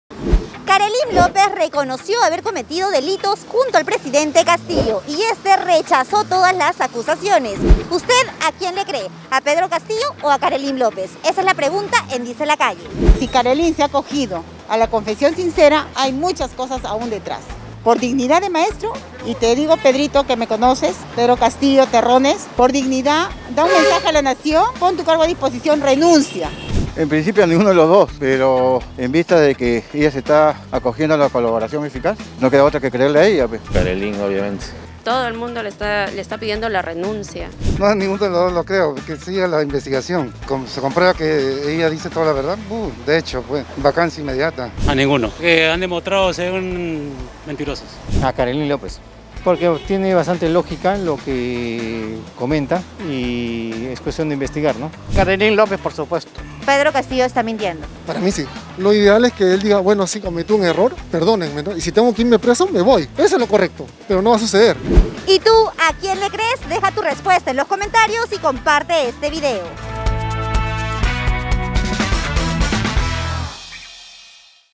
🚨📣 ¿A quién le cree usted a Karelim López o al presidente Pedro Castillo?
🚨📣 DiceLaCalle | KarelimLópez reconoció haber cometido delitos junto al presidente PedroCastillo y él rechazó todas las acusaciones.
¿A quién le creemos?
🔴 Notas Comentadas

📺 18.1 señal digital abierta